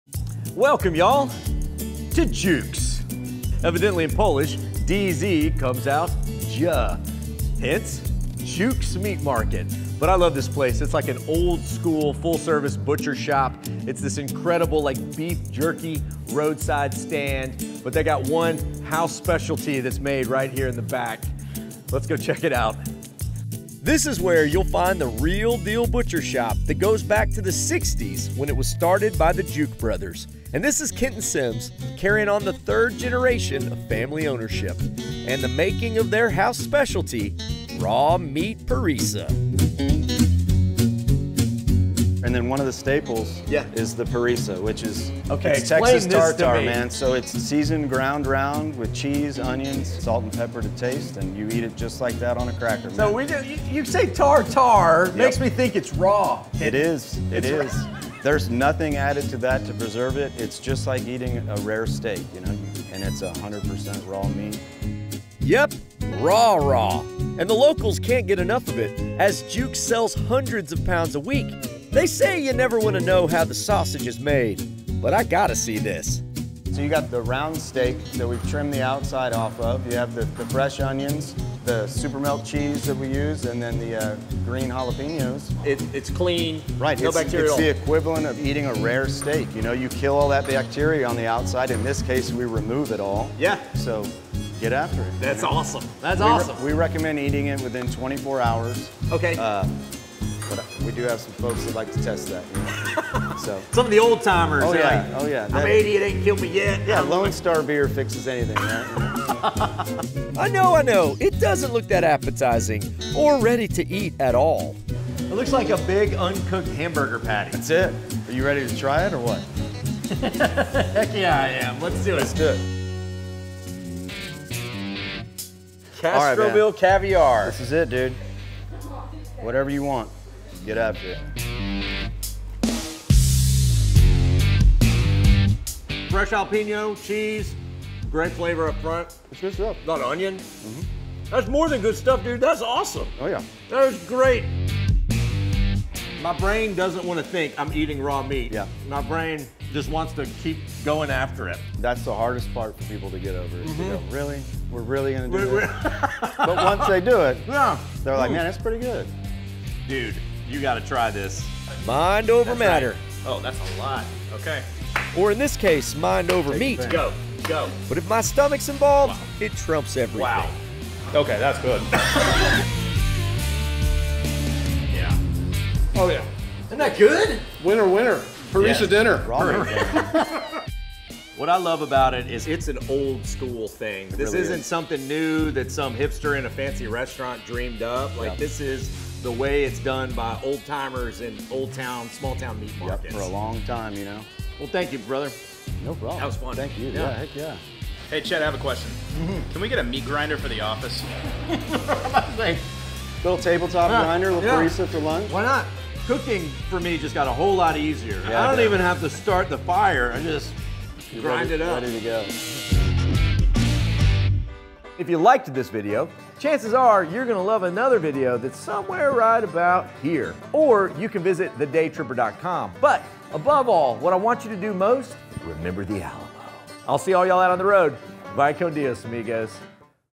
Raw Meat "Parisa" a Texas tradition from Dziuk's in Castroville, TX
I'm a sucker for a good rare cooked steak, but I don't know about RAW MEAT! But at Dziuk's Meat Market, raw meat is the key ingredient in their most popular menu item...Parisa. I'm here to try it to see what all the fuss is about.